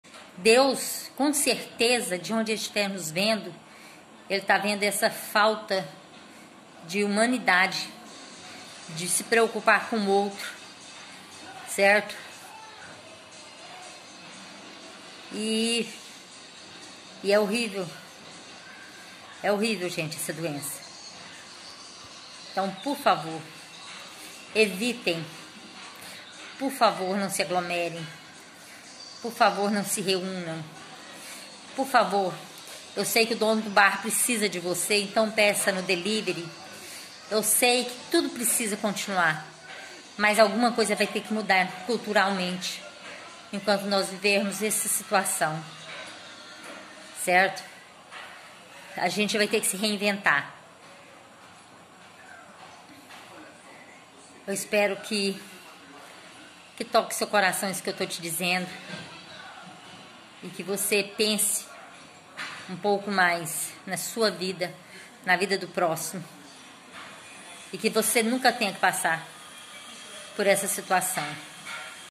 PROFESSORA NORMA BONTEMPO PEDE PARA NÃO AGLOMERAR
A professora Norma Bontempo, positivada para covid-19, gravou vídeo pedindo para as pessoas evitarem aglomerar para combater o Covid-19, em Jussara, 27/07/2021.